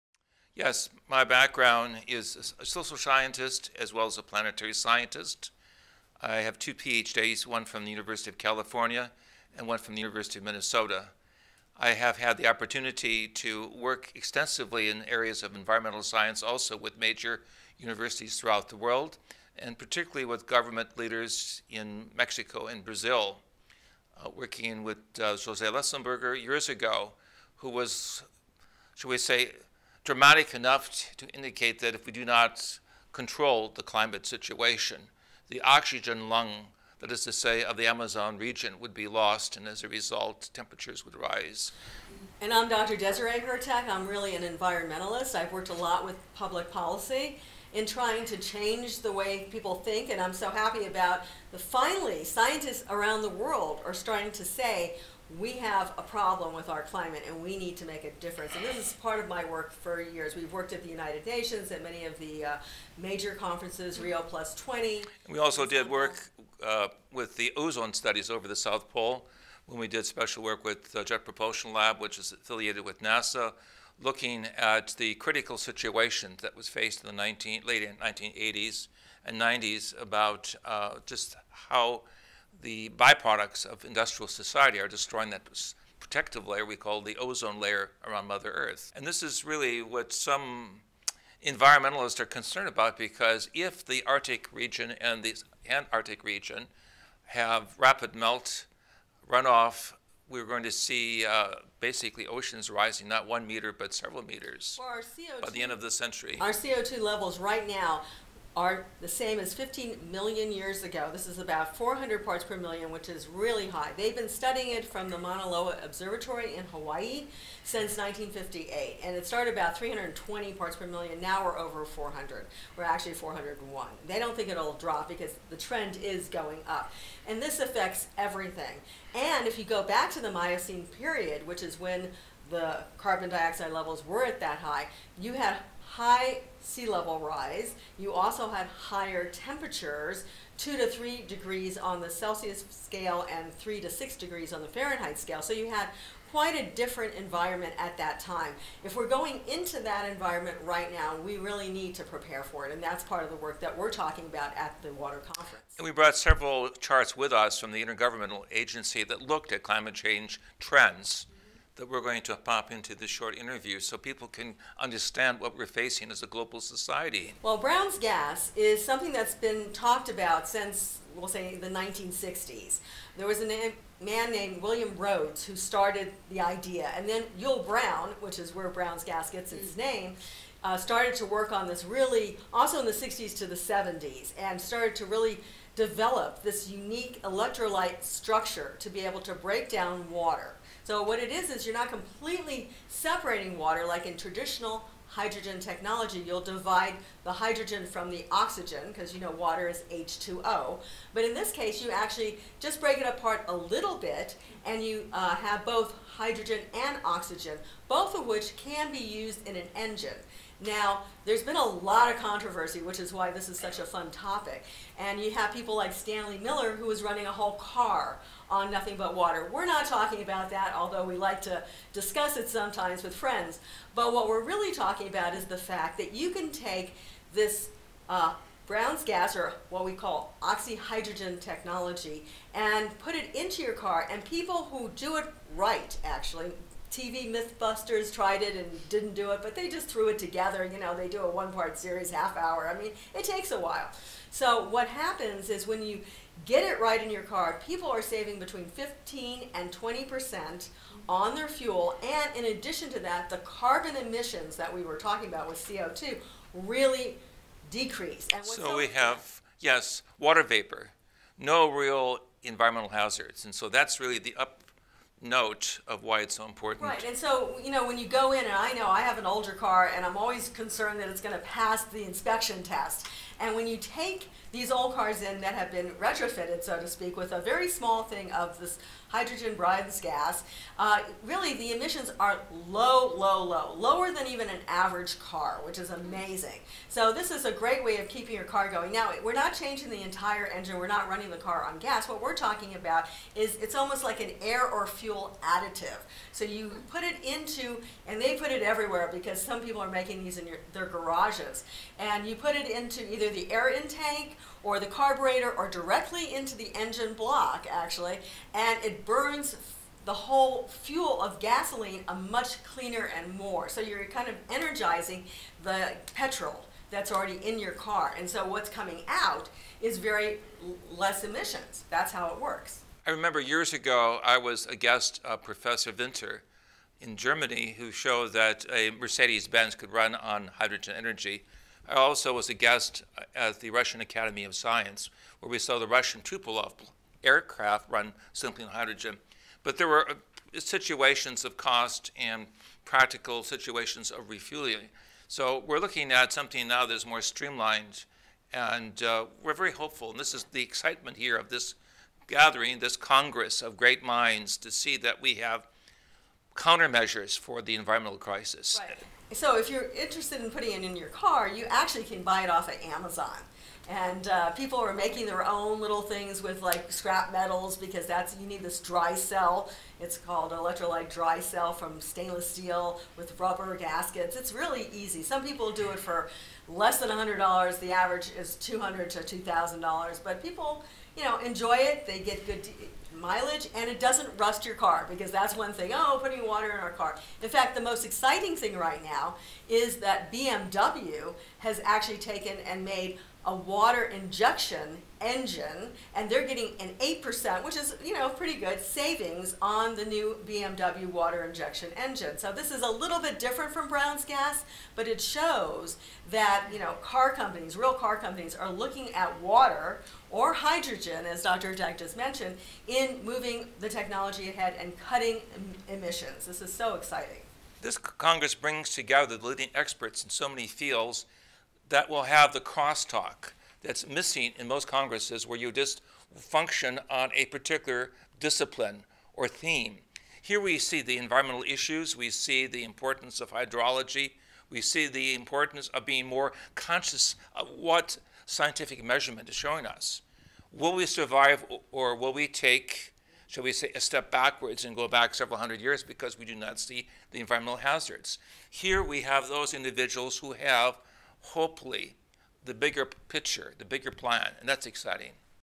James & Desiree Hurtak Interview
OxyHydrogen: An Examination of Brown’s Gas

James Joachim Hurtak and Desiree Hurtak, Academy for Future Science
James is a social scientist and researcher who, along with his wife, an environmentalist, has researched energy technologies throughout the world. They are the founders of The Academy For Future Science which is an international organization working around the world to better our environment.